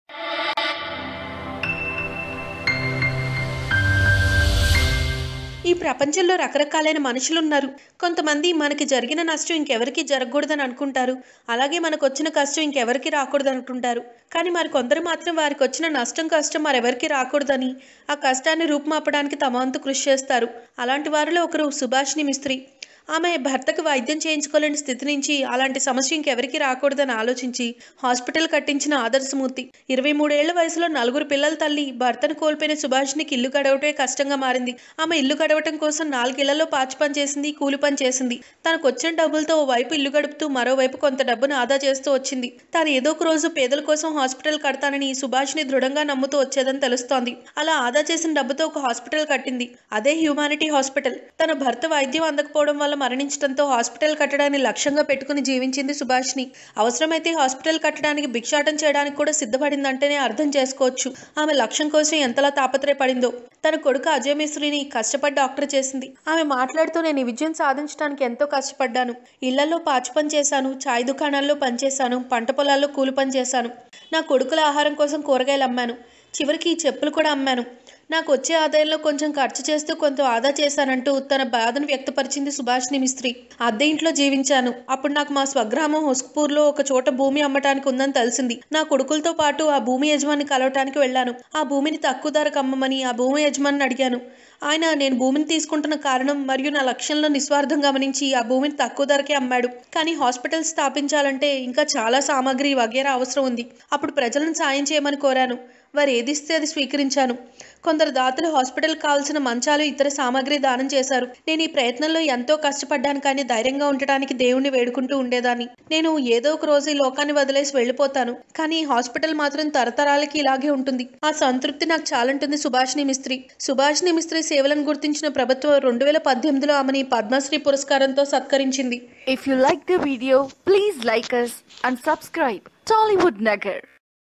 Poor Woman Built Hospital For Poor People | Subhasini Mistry Welfare Activities | Tollywood Nagar
Poor Woman Built Hospital For Poor People, Indian Social Worker Subhasini Mistry's has built a hospital named Humanity Hospital in the memory of her late husband, watch out this complete video to know it better.

Hey guys!